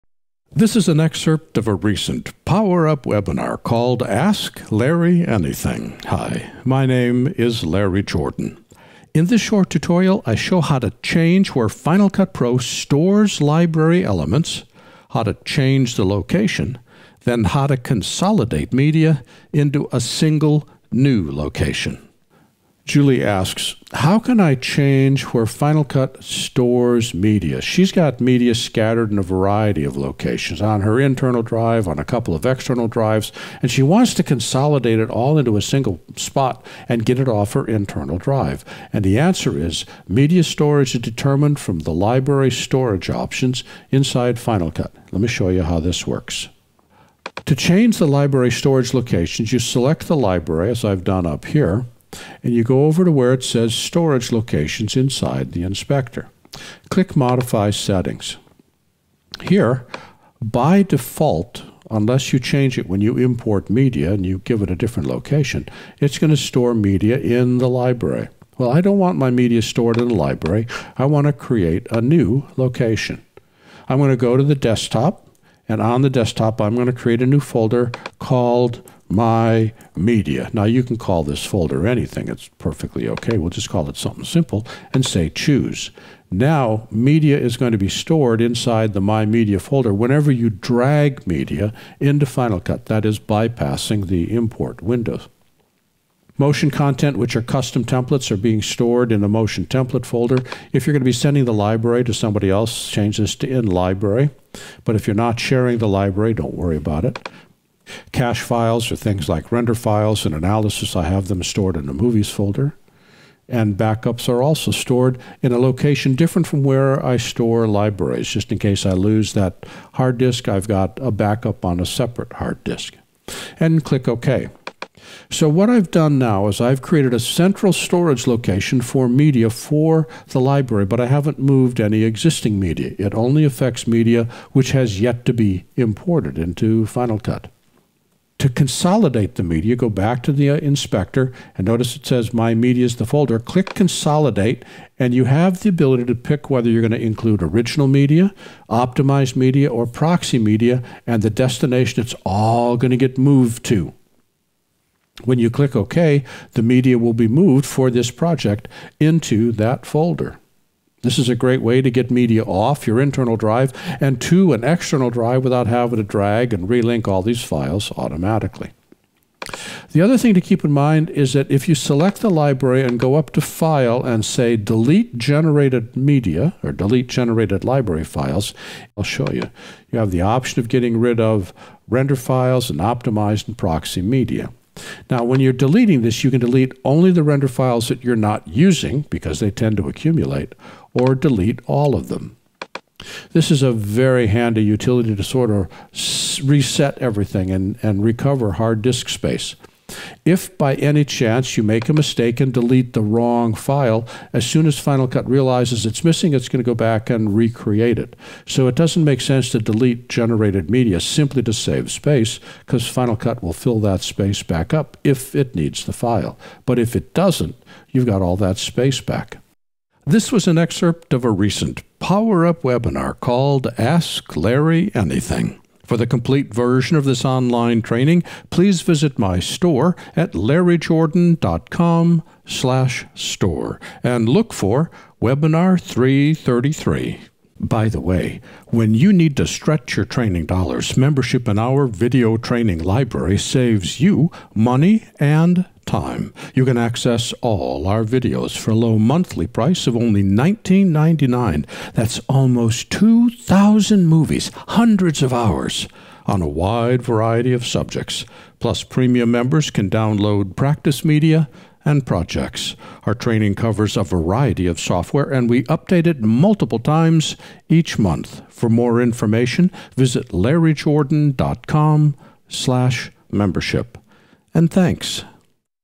How To Change Final Cut Pro Library Storage Locations
"How To Change Final Cut Pro Library Storage Locations" is an excerpt from a recent PowerUp webinar - "Ask Larry Anything!" - presented by Larry Jordan.

This is an excerpt of a recent PowerUP webinar called "Ask Larry Anything." In this short tutorial, Larry Jordan shows how to change where Final Cut Pro stores library elements, how to change that location, then how consolidate media into a single new location.